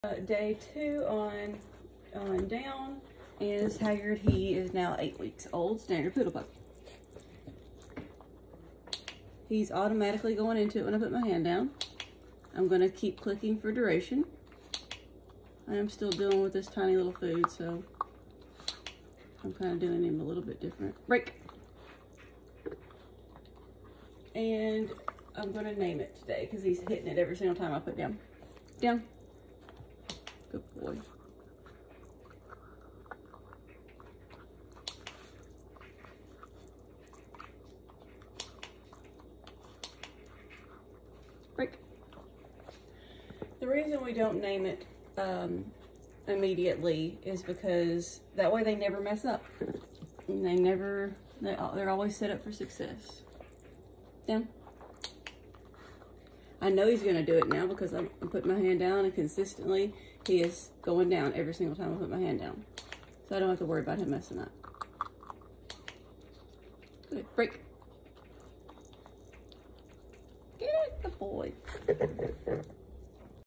Down-dog training Day2 Haggard
8 week standard poodle puppy Haggard